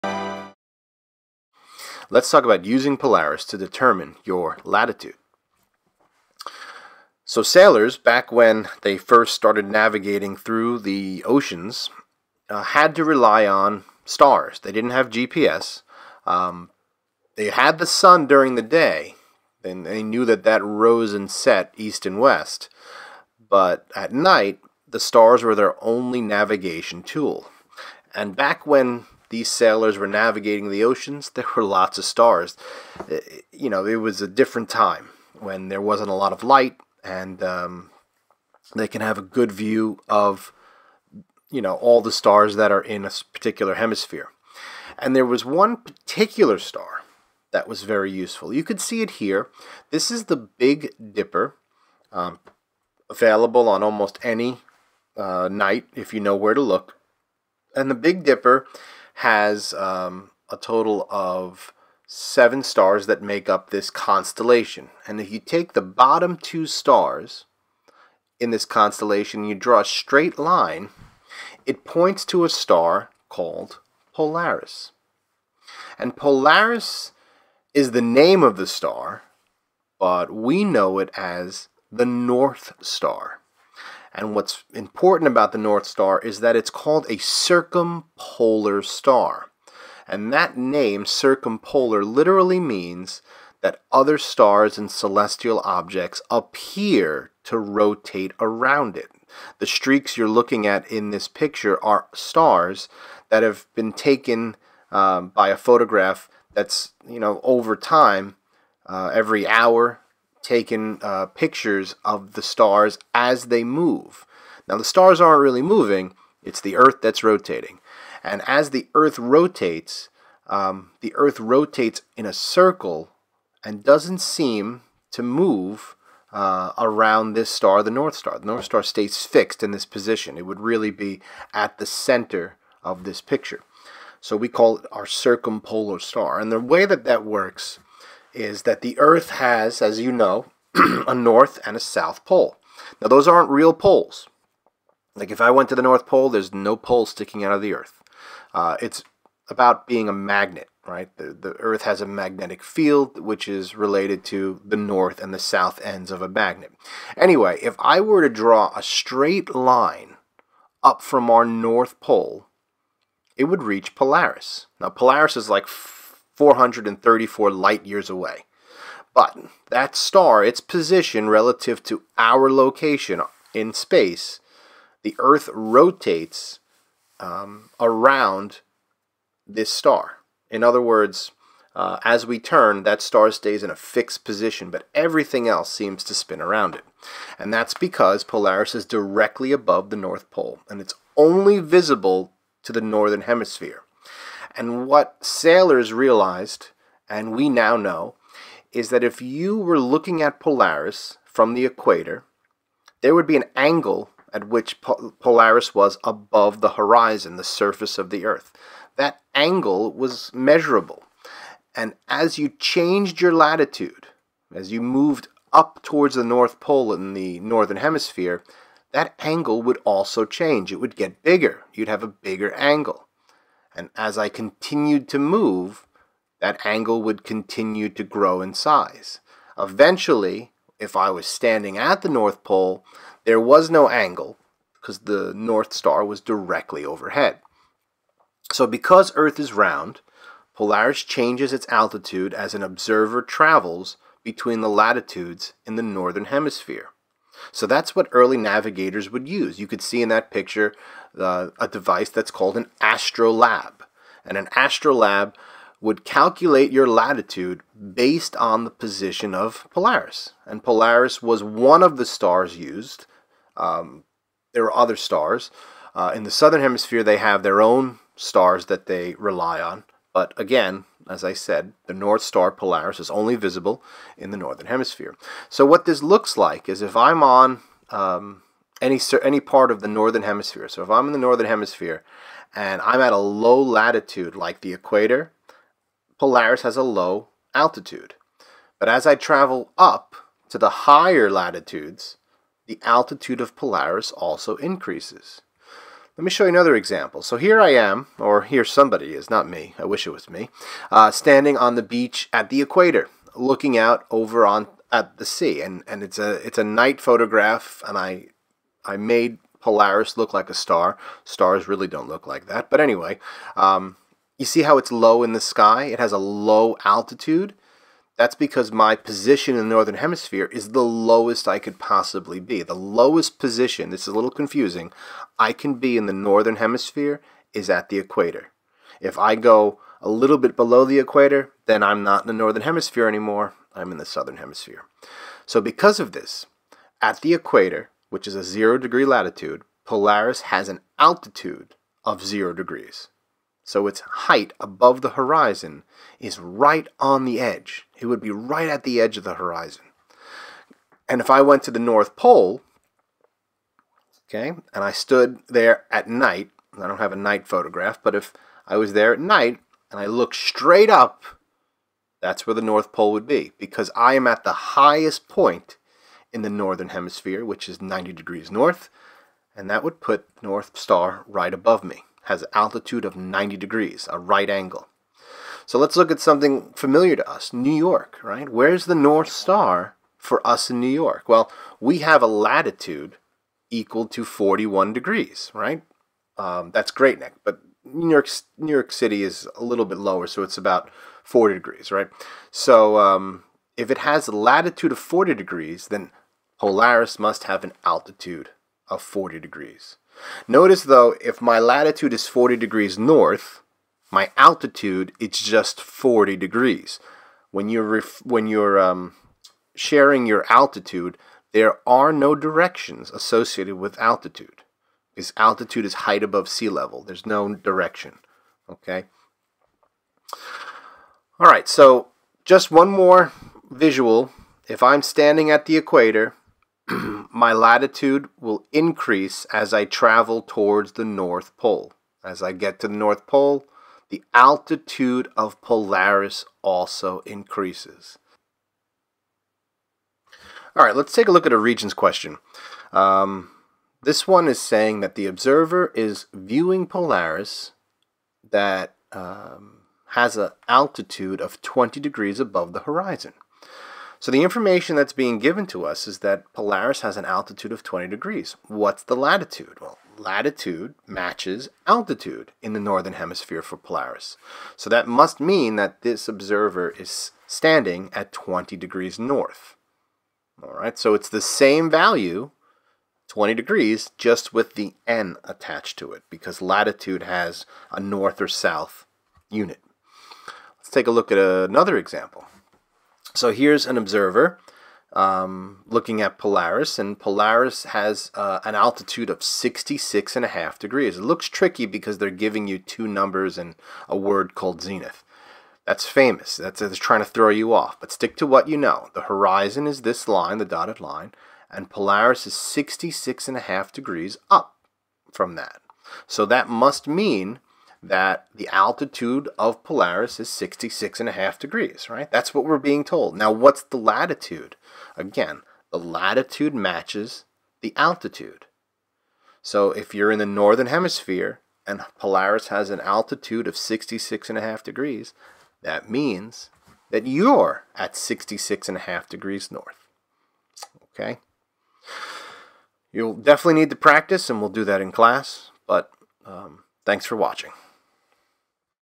Earth Science - Using Polaris to calculate Latitude
This video explores the relationship between Polaris and latitude.